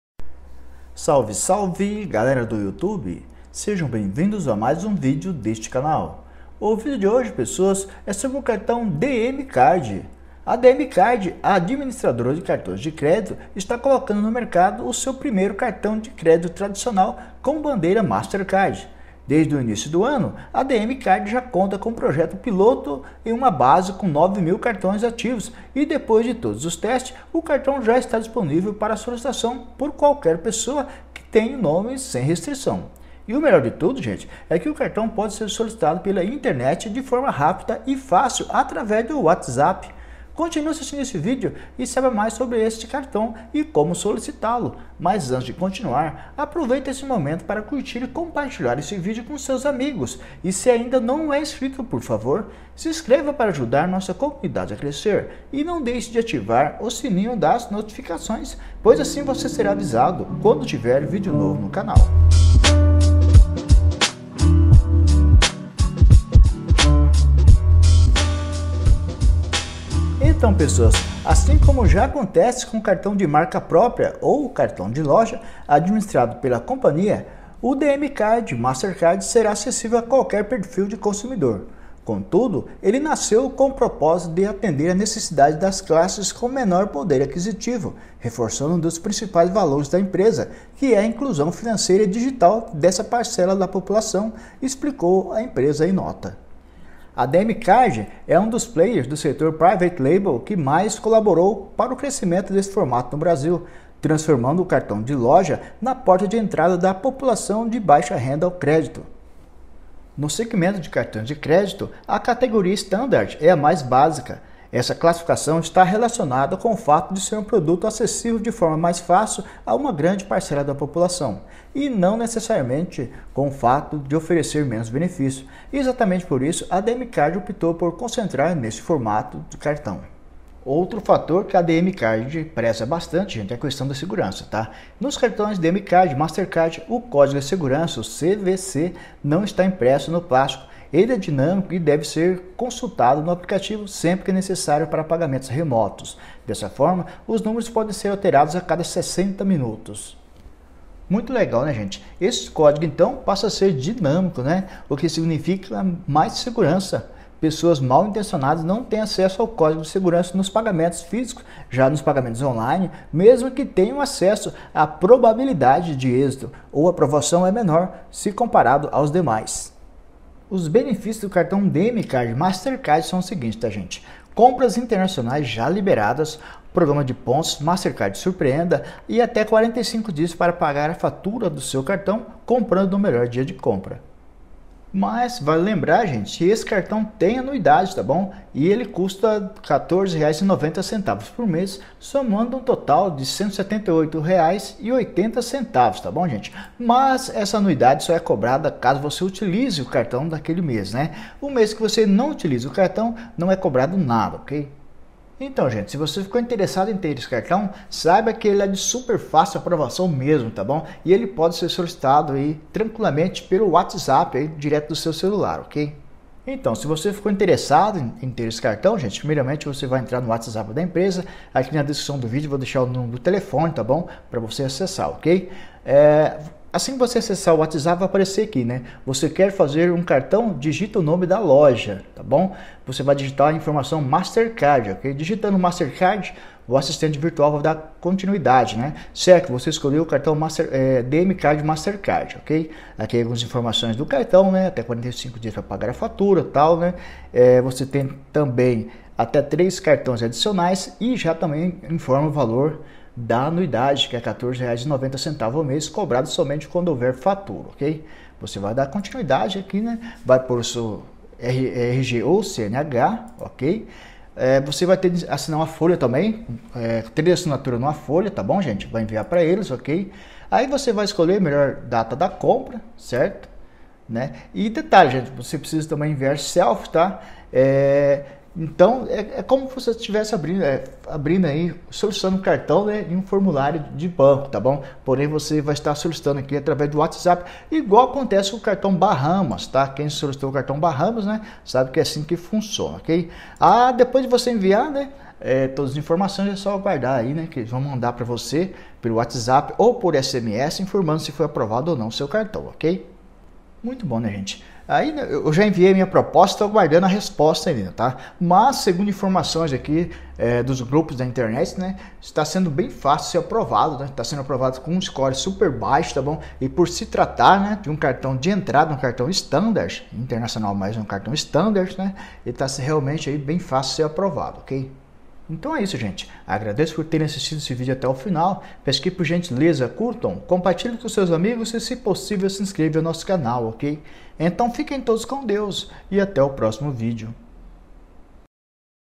Novo cartão Dm Card Mastercard Internacional fácil aprovação não precisa comprovar renda
A DMCard, administradora de cartões de crédito, está colocando no mercado o seu primeiro cartão de crédito tradicional com a bandeira Mastercard.
Já conhece os benefícios do cartão?
✔ Compras internacionais já liberadas;
✔ Programa de pontos Mastercard®️ Surpreenda;
✔ Até 45 dias para pagar sua fatura, comprando no melhor dia de compra;
Para solicitar o cartão
Para saber mais sobre o Dm Card entre no link abaixo;